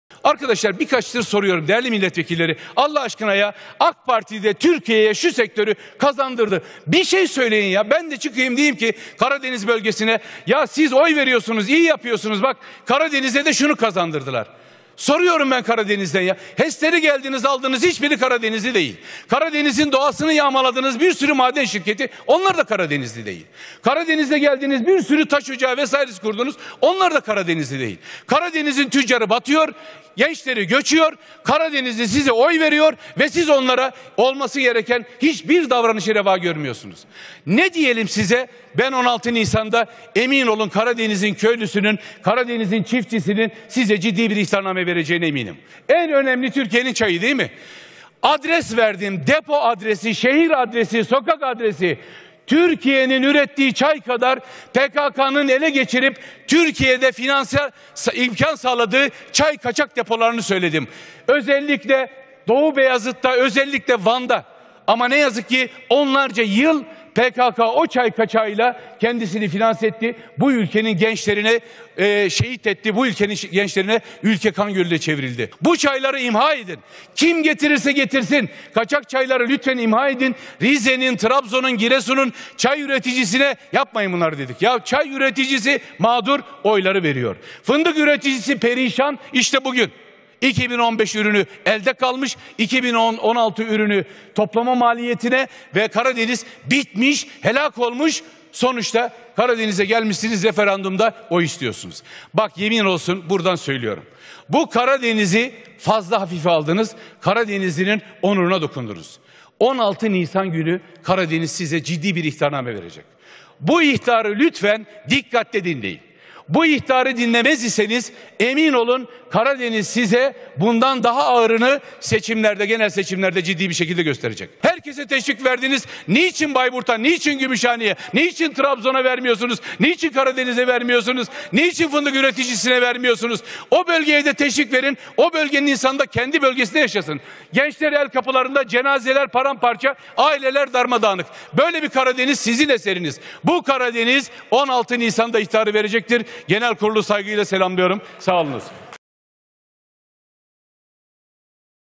Karadeniz Köylüsüyle, Çiftçisiyle 'Hayır' Diyecek!
Trabzon Milletvekilimiz Haluk Pekşen:  Karadeniz'e HES'leri getirdiniz, doğasını yağmaladınız, bir sürü maden şirketi, bir sürü taş ocağı kurdunuz. Karadeniz'in tüccarı batıyor, gençleri göçüyor. Karadeniz bitmiş, helak olmuş; gelmişsiniz, referandumda oy istiyorsunuz. Bu Karadeniz'i fazla hafife aldınız. 16 Nisan günü Karadeniz size ciddi bir ihtarname verecek!